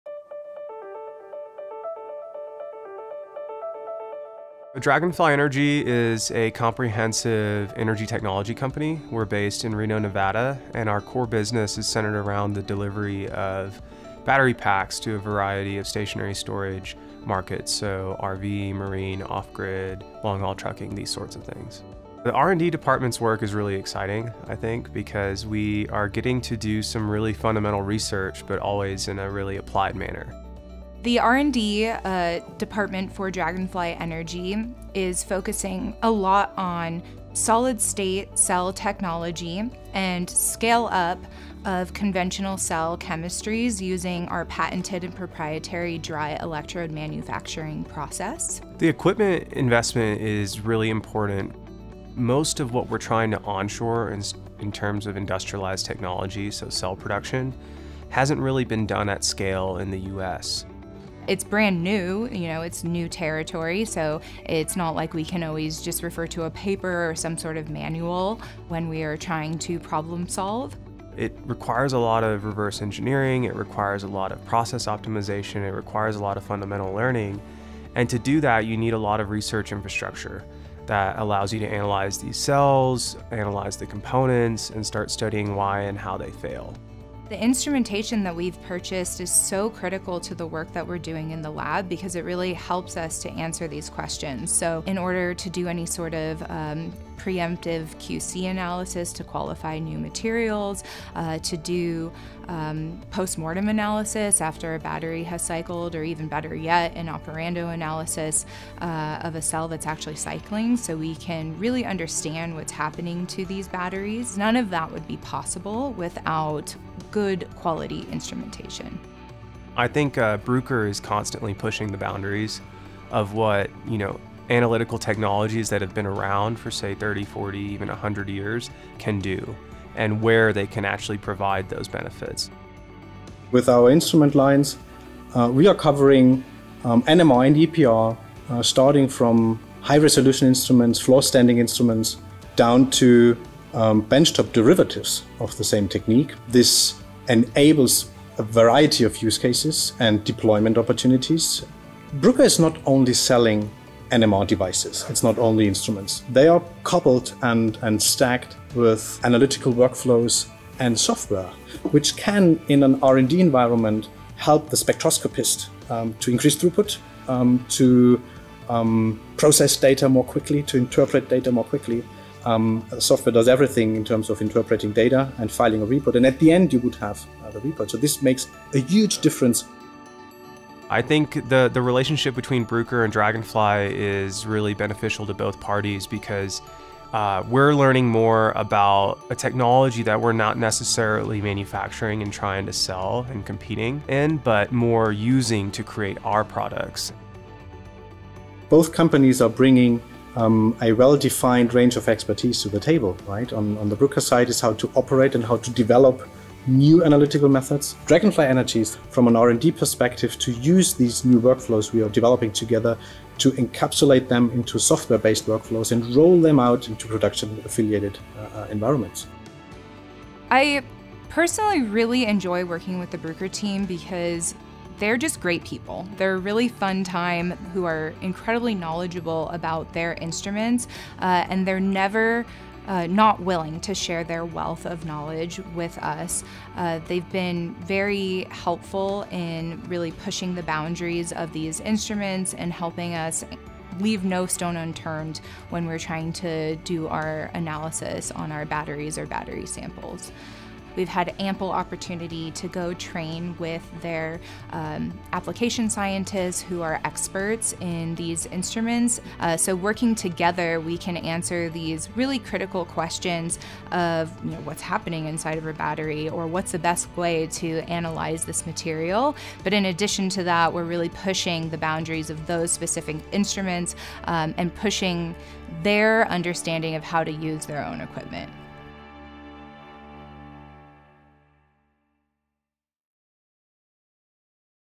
Bruker’s Cutting-Edge Instrumentation Paving the Way for Dragonfly Energy's Lithium Battery Research
As a comprehensive green energy technology company, Dragonfly Energy's Research & Development initiatives and team play an integral role in pushing the boundaries of fundamental lithium battery research. Utilizing state-of-the-art equipment through our partnership with @Bruker, a leading manufacturer of high-performance scientific instruments and analytical and diagnostic solutions, Dragonfly Energy scientists have been able to perform comprehensive analyses of lithium and solid state cell technology, our patented and proprietary dry electrode manufacturing process, and much more, all at a fundamental level.

Working closely with Bruker has allowed Dragonfly Energy to complete innovative work in the lab with the use of their instrumentation and technology, and with these cutting-edge tools and this valuable partnership, we will continue to pave the way for the future of green energy solutions.

Hear from Dragonfly Energy and Bruker in this video about the notable work we've been able to do together.
Dr. Vick Singh, SVP of Technology at Dragonfly Energy (Formerly Director of R&D)
Emily Litt, R&D Scientist, Group Lead at Dragonfly Energy
Dr. Joerg Koehler, Head of Industrial Business Unit at Bruker

Learn more about the incredible science research and diagnostics solutions Bruker is developing to enable scientists to make breakthrough discoveries